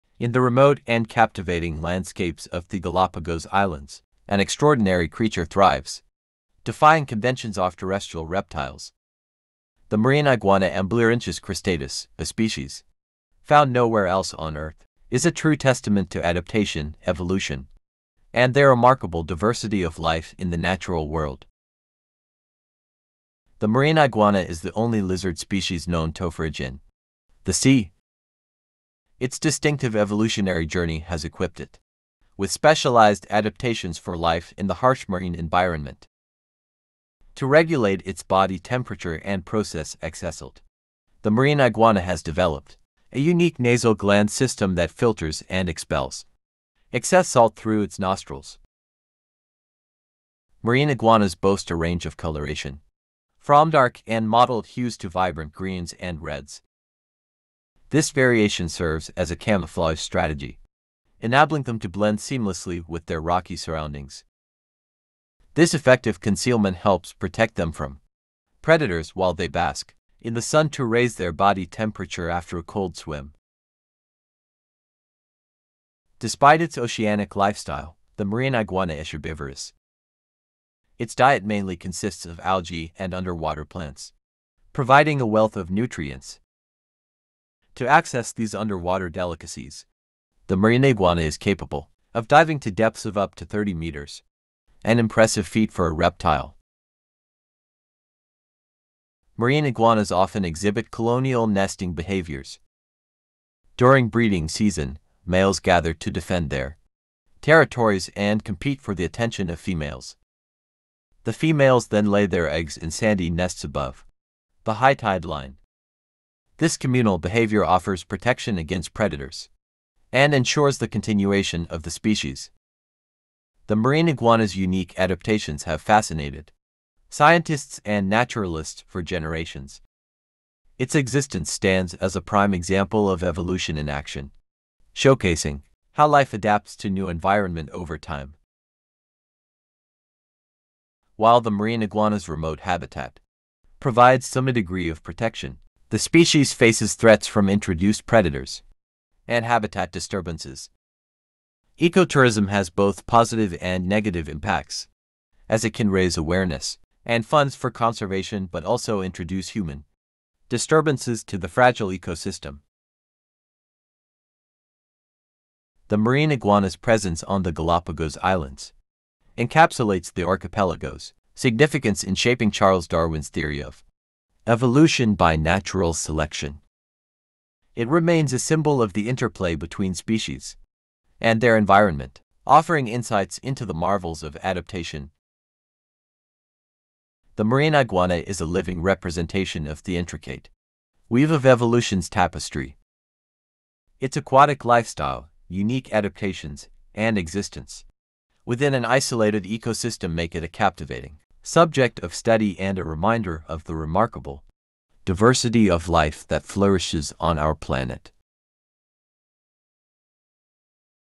Diving into the Secrets of the Marine Iguana: Galápagos Marvels
Explore the fascinating world of the marine iguana, a unique reptile that defies convention by foraging in the sea. Discover its aquatic adaptations, coastal camouflage, and role in the Galápagos ecosystem. Join us as we uncover the remarkable story of evolution, conservation challenges, and the enigmatic allure of this iconic creature. 🌊🦎